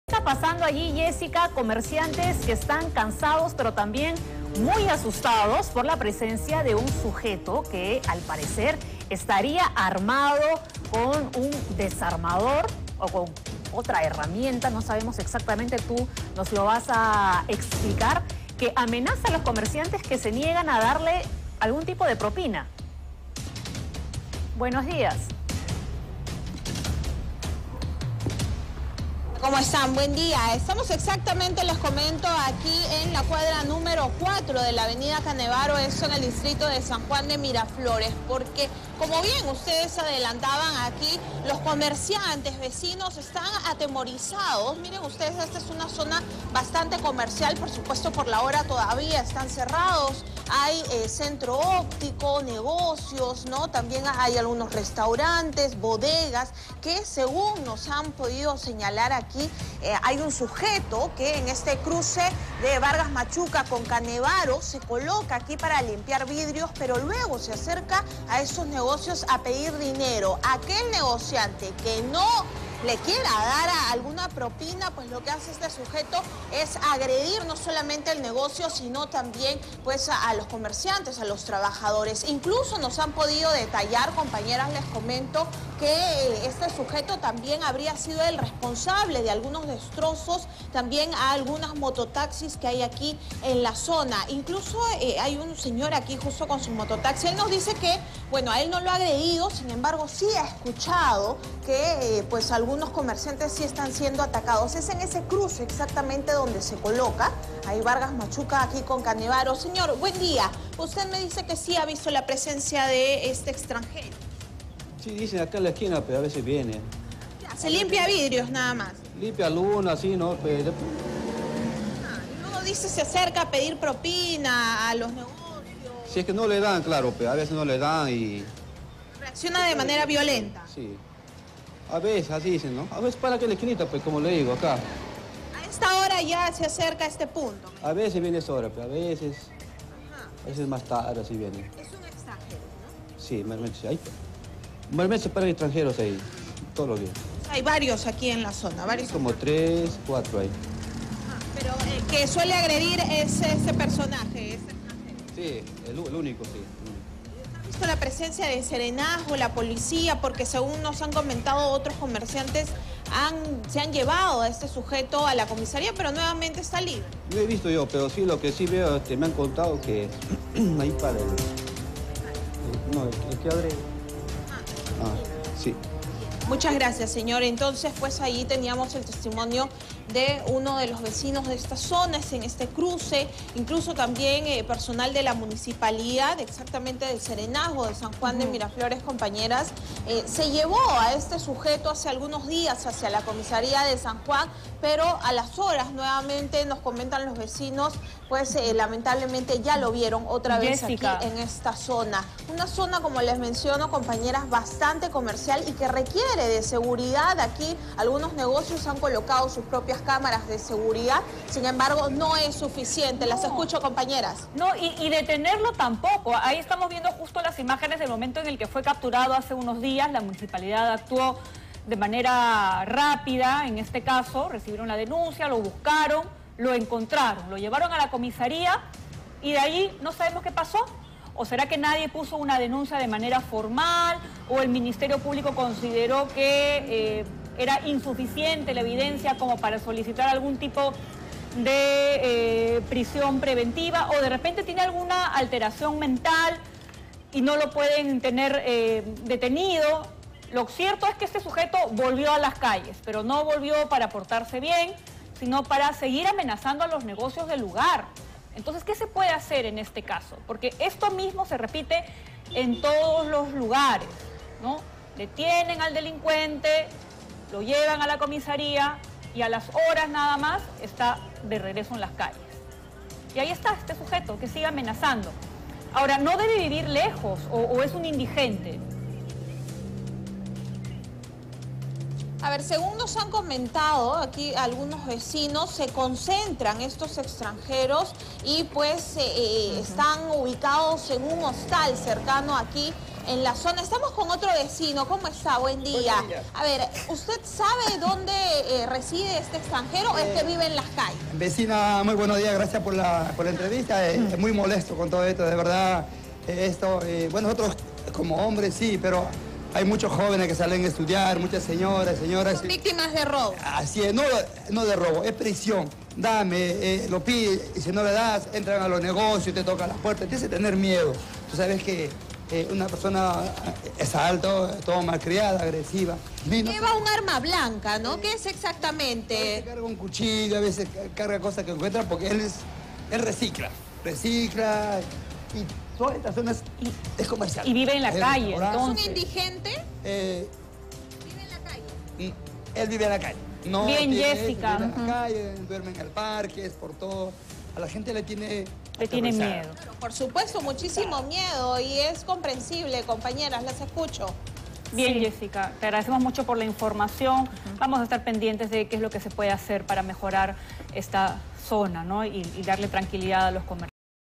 SJM: hombre que limpia vidrios ataca a los comerciantes que no le dan propina
Los vecinos de la avenida Canevaro en el distrito de San Juan de Miraflores denuncian que un sujeto se encuentra atacando con un desarmador a los comerciantes de la zona que se niegan a darle propina.

Según los residentes, este hombre extranjero se coloca entre la avenida Vargas Machuca con Canevaro para limpiar los videos de los autos que transitan por la vía.

DESTROZOS

Los residentes denuncian que no solo han atacado a los comerciantes, sino que también han atentado contra los mototaxistas.

Hace pocos días, personal de serenazgo del distrito se llevó a este sujeto hacia la comisaría, pero fue liberado a las horas.